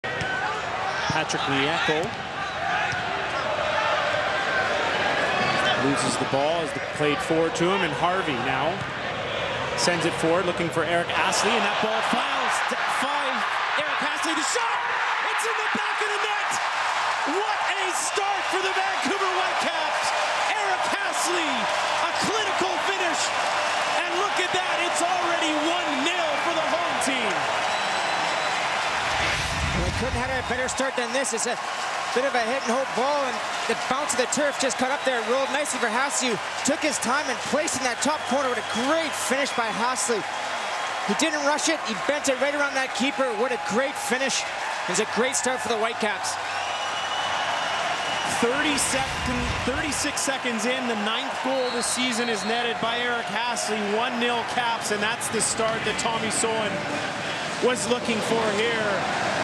Vancouver Whitecaps FC vs Chicago Fire - Eric Hassli 1-0 Goal - 2011.08.07 - HD
Eric Hassli opens the scoring less than a minute into the match.